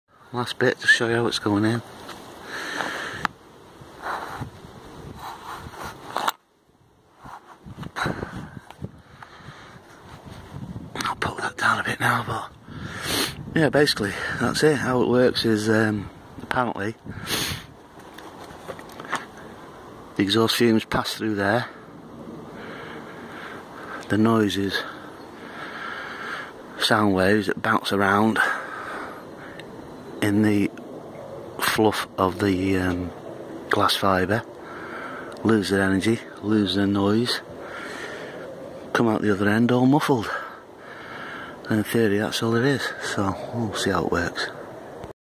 Custom exhaust no 5
Sliding the Baffle in the exhaust